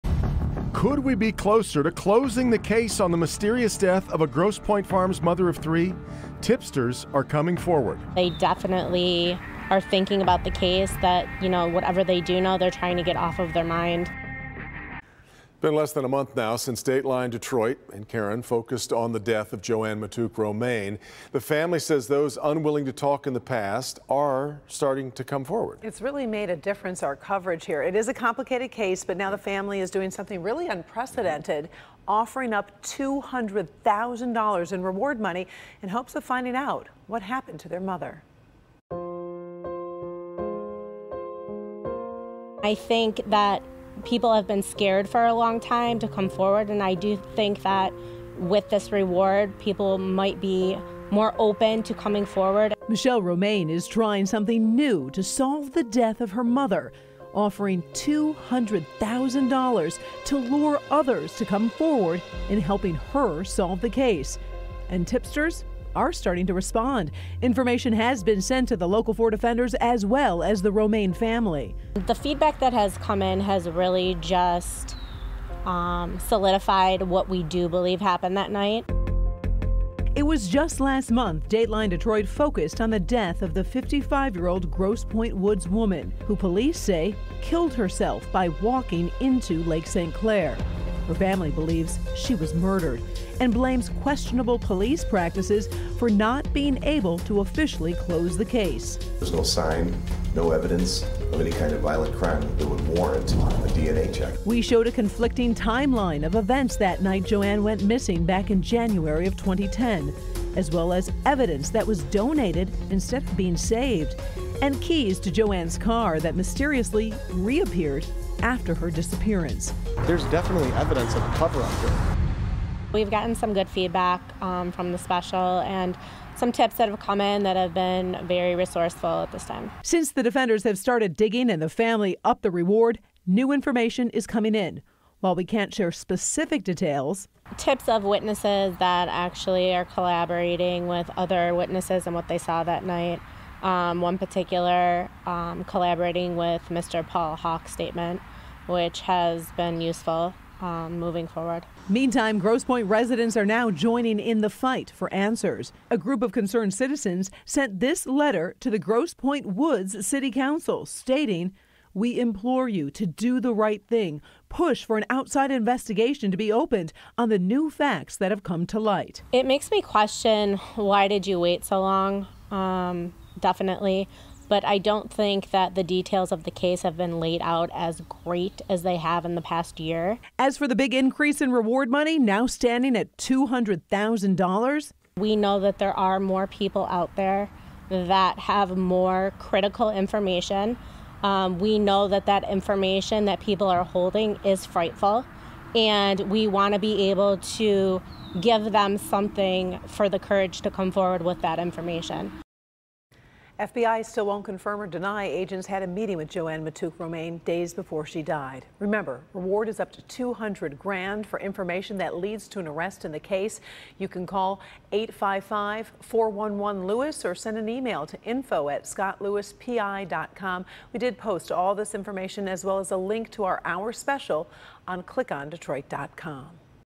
$200k reward offered for tips in death of JoAnn Matouk Romain
It’s been less than a month since Dateline Detroit focused on the death of JoAnn Matouk Romain and the family said people unwilling to talk in the past are starting to come forward.